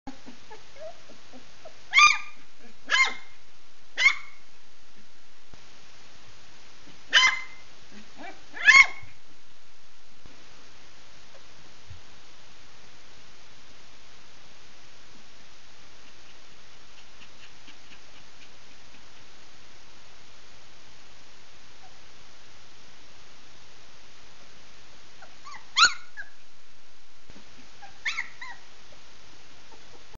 Les bébés de Ulane et Nousky
Les bébés de Ulane et Nousky en pleine action, enfin pas tous car deux dont mon fils restent à se reposer dans un coin!